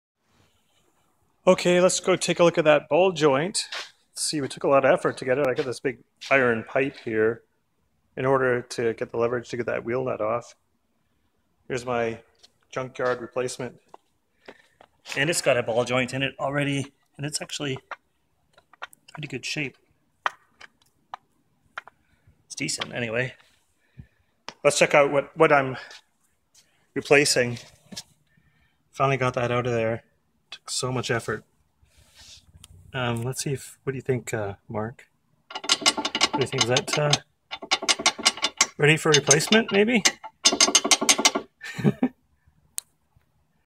Result of front ball joint replacement on 2000 Toyota Echo.
Replaced a ball joint that would not succumb to ball joint removal techniques and special tools. I removed the wheel nut and unbolted the strut and tie rod to swing the knuckle out of the way so I could pound out the ball joint from above.